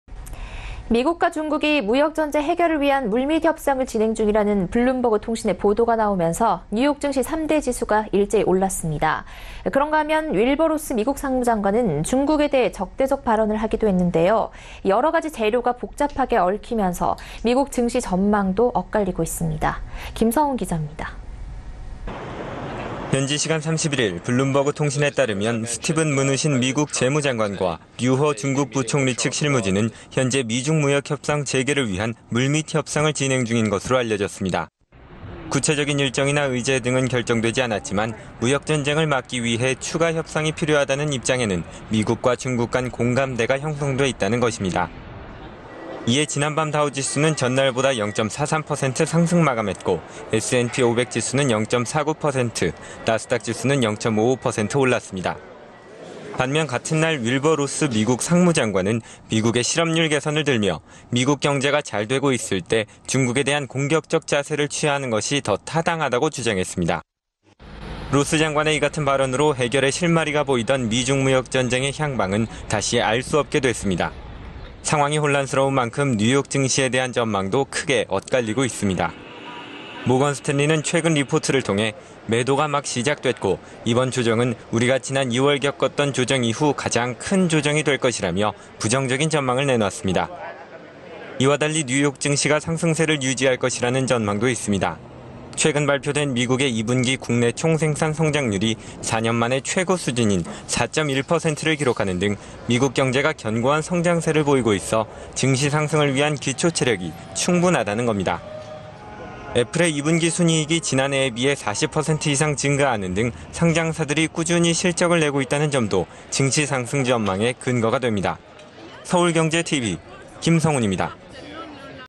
[서울경제TV] 미·중 무역전쟁 향방 혼란... 美 증시 전망도 엇갈려
[앵커]
미국과 중국이 무역전쟁 해결을 위한 물밑협상을 진행 중이라는 블룸버그통신의 보도가 나오면서 뉴욕증시 3대 지수가 일제히 올랐습니다. 그런가 하면 윌버 로스 미국 상무장관은 중국에 대해 적대적 발언을 하기도 했는데요. 여러 가지 재료가 복잡하게 얽히면서 미국 증시 전망도 엇갈리고 있습니다. 김성훈기자가 보도합니다.

[기자]
현지시간 31일, 블룸버그통신에 따르면 스티븐 므누신 미국 재무장관과 류허(劉鶴) 중국 부총리 측 실무진은 현재 미·중 무역협상 재개를 위한 물밑협상을 진행 중인 것으로 알려졌습니다.
구체적인 일정이나 의제 등은 결정되지 않았지만, 무역전쟁을 막기 위해 추가 협상이 필요하다는 입장에는 미국과 중국 간 공감대가 형성돼 있다는 것입니다.
이에 지난 밤 다우지수는 전날보다 0.43% 상승 마감했고, S&P500지수는 0.49%, 나스닥지수는 0.55% 올랐습니다.

반면 같은 날 윌버 로스 미국 상무장관은 미국의 실업률 개선을 들며 “미국 경제가 잘되고 있을 때 중국에 대한 ‘공격적 자세’를 취하는 것이 더 타당하다”고 주장했습니다.
미국의 실업률은 현재 4% 수준으로 사실상 완전고용 상태를 유지하고 있습니다.

로스 장관의 이 같은 발언으로 해결의 실마리가 보이던 미·중 무역전쟁의 향방은 다시 알 수 없게 됐습니다.

상황이 혼란스러운 만큼 뉴욕 증시에 대한 전망도 크게 엇갈리고 있습니다.
모건스탠리는 최근 리포트를 통해 “매도가 막 시작됐고, 이번 조정은 우리가 지난 2월 겪었던 조정 이후 가장 큰 조정이 될 것”이라며 부정적인 전망을 내놨습니다.
“상장사들의 실적이 좋음에도 경기 민감주의 주가가 예상보다 오르지 못하고 있는데, 이것이 증시 랠리가 동력을 다했다는 신호일 수 있다”는 분석입니다.

이와 달리 뉴욕 증시가 상승세를 유지할 것이라는 전망도 있습니다.
최근 발표된 미국의 2분기 국내총생산(GDP) 성장률이 4년 만에 최고 수준인 4.1%를 기록하는 등 미국 경제가 견고한 성장세를 보이고 있어 증시 상승을 위한 기초체력이 충분하다는 것입니다.
애플의 2분기 순이익이 지난해에 비해 40% 이상 증가하는 등 상장사들이 꾸준히 실적을 내고 있다는 점도 증시 상승 전망의 근거가 됩니다.